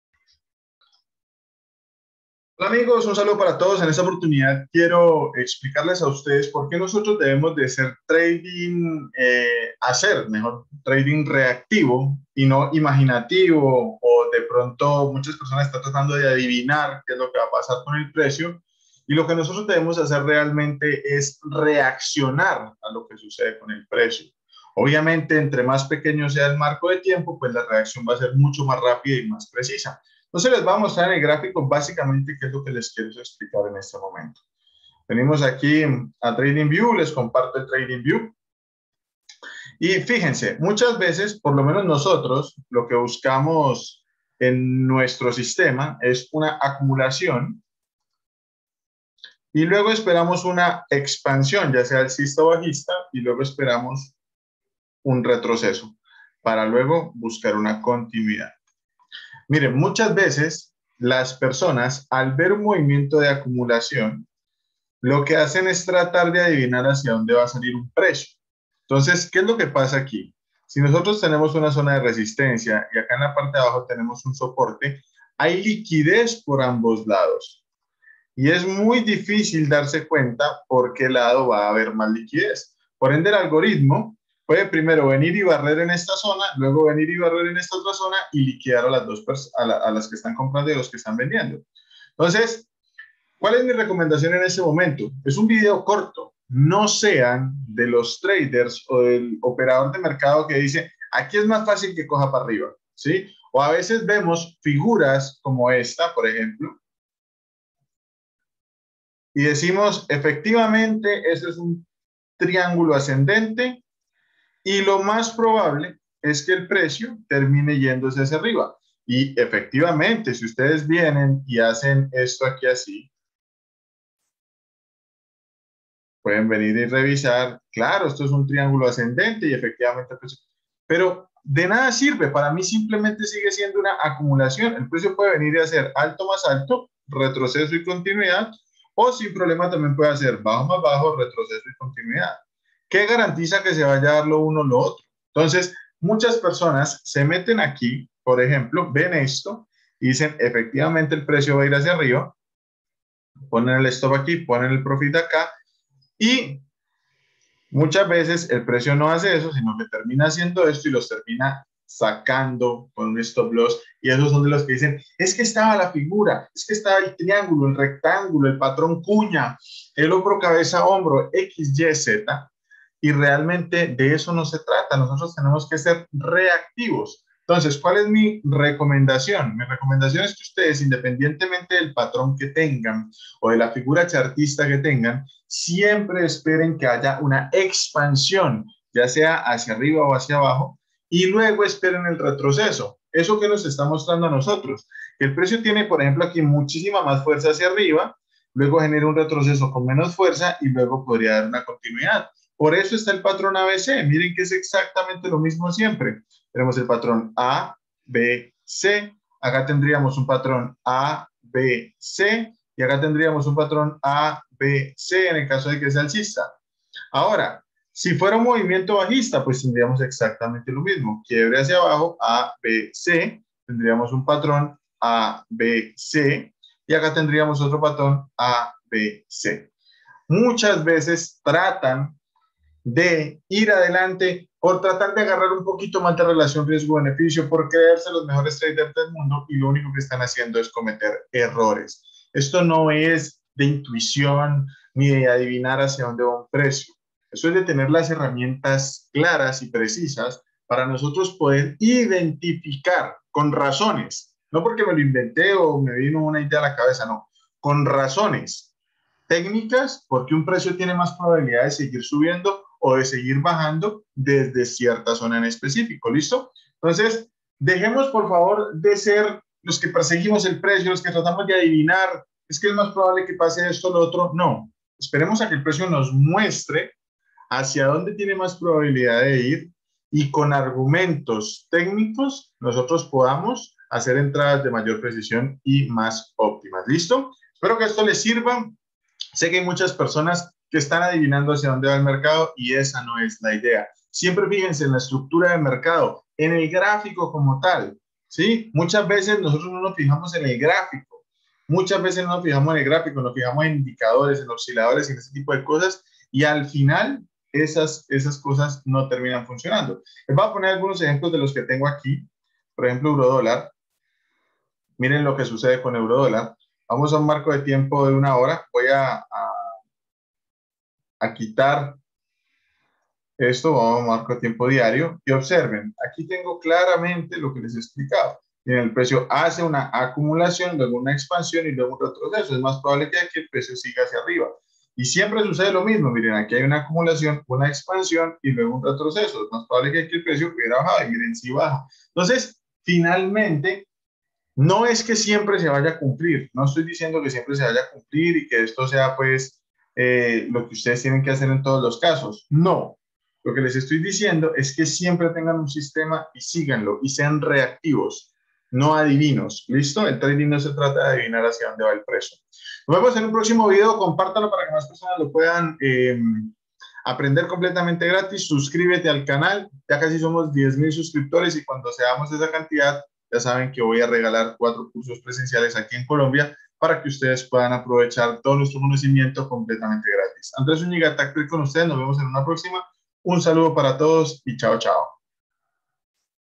No adivines la dirección del precio, reacciona!!
Lo mejor es esperar que le precio nos indique cual es nueva dirección y cual es la mayor probabilidad, no debemos nosotros adivinar antes de que esto pase, el estar adivinando solo nos lleva a tener mas perdidas y no saber porque perdemos, sino seguimos un plan no podremos saber que esta fallando y en donde tenemos que mejorar. Saludos AZ